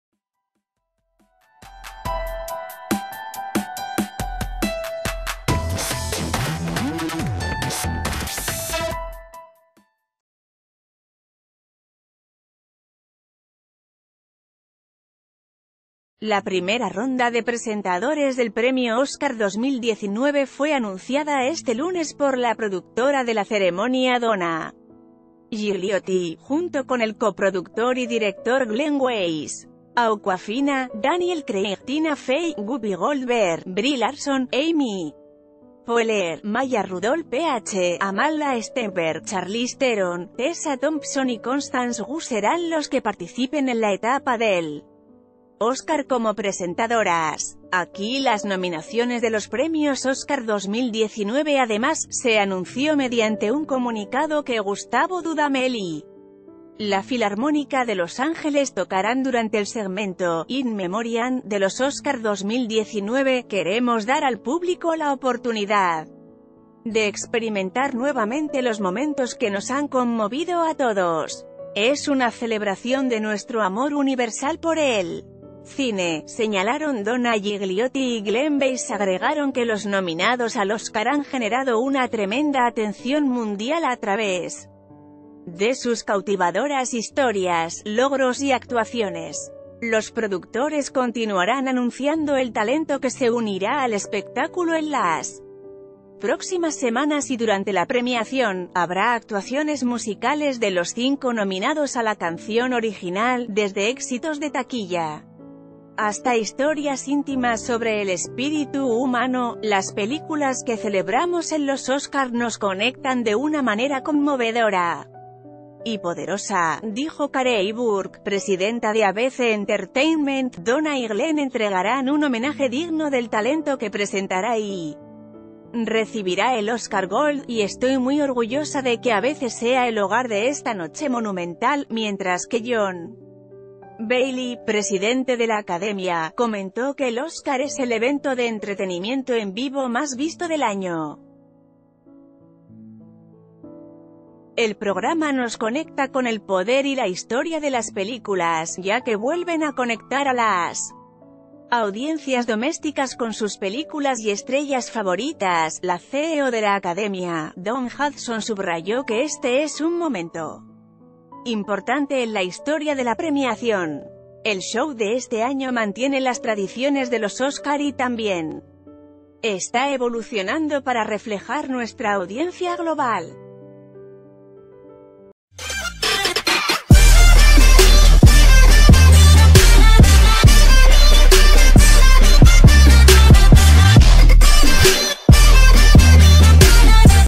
Ellos son los primeros presentadores de los premios Oscar
La primera ronda de presentadores del premio Oscar 2019 fue anunciada este lunes por la productora de la ceremonia Donna Gigliotti, junto con el coproductor y director Glenn Weiss. Awkwafina, Daniel Craig,, Tina Fey, Whoopi Goldberg, Brie Larson, , Amy Poehler, Maya Rudolph, Amandla Stenberg, Charli...